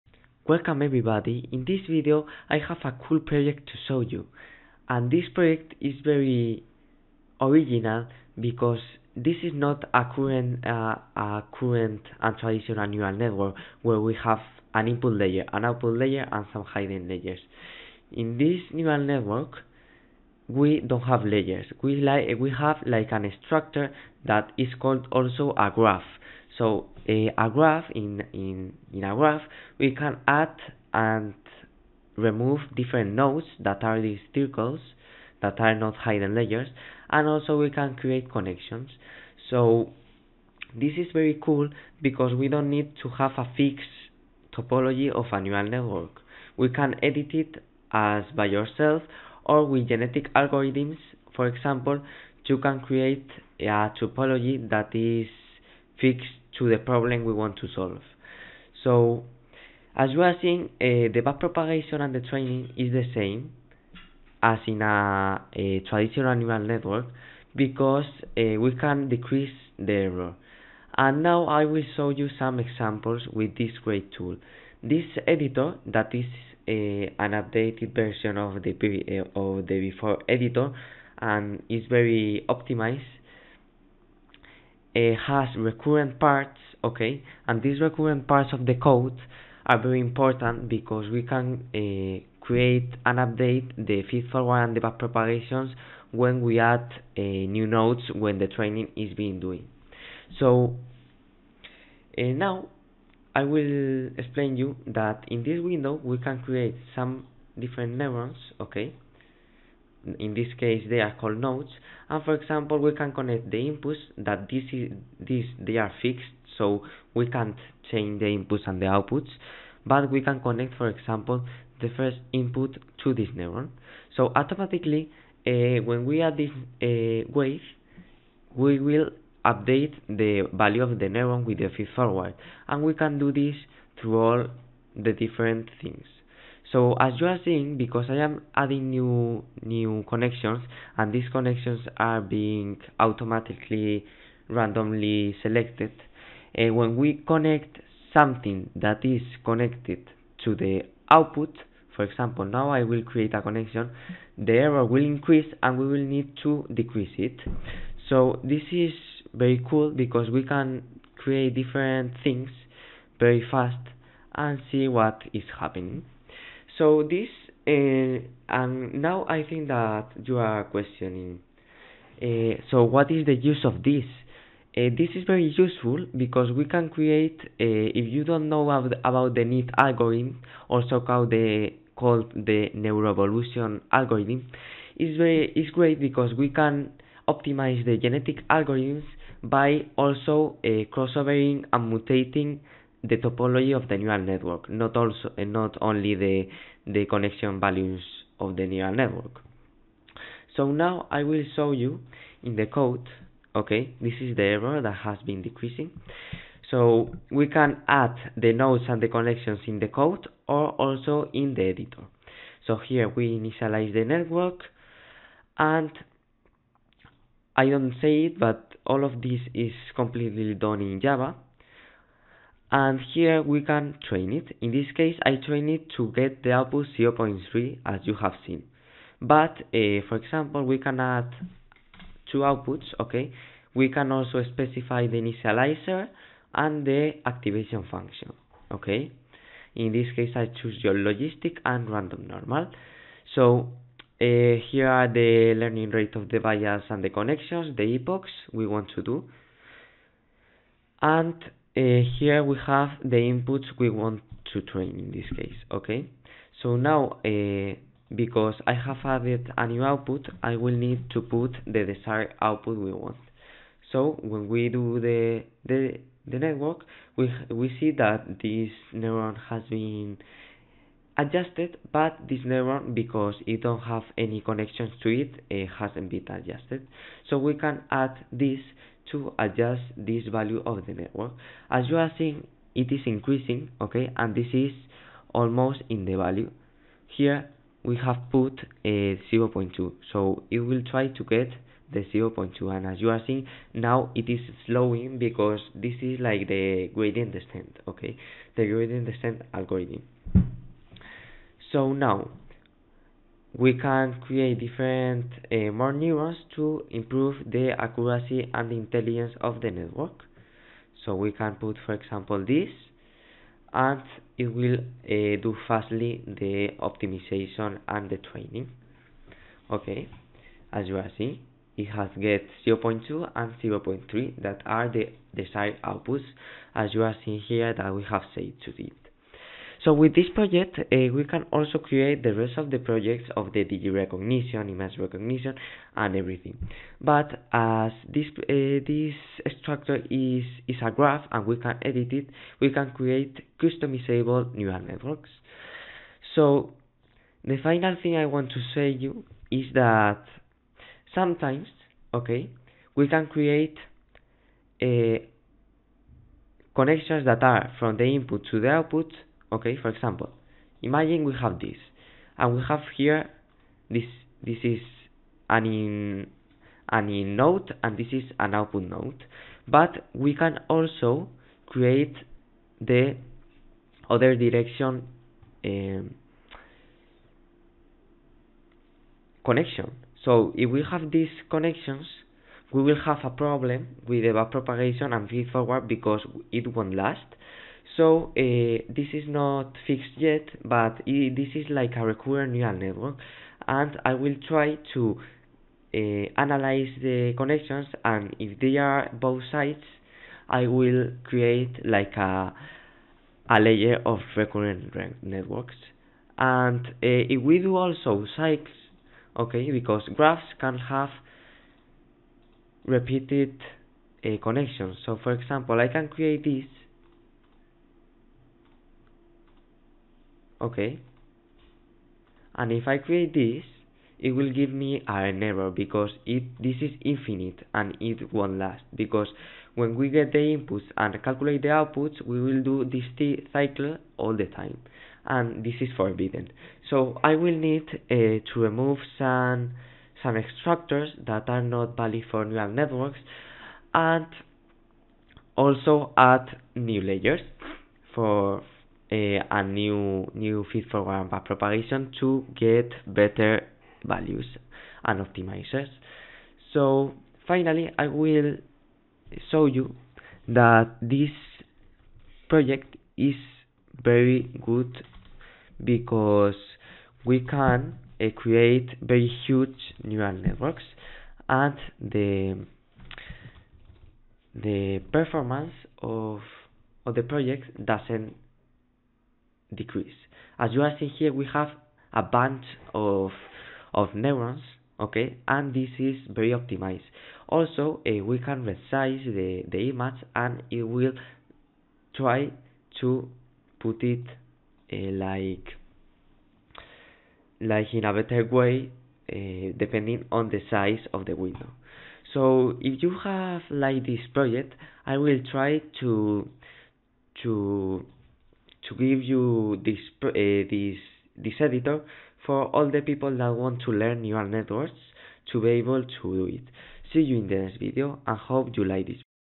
Evolving the topology of a neural network - Neuroevolution Neural Network Editor
Cool projects of neuroevolution of neural networks in the training proccess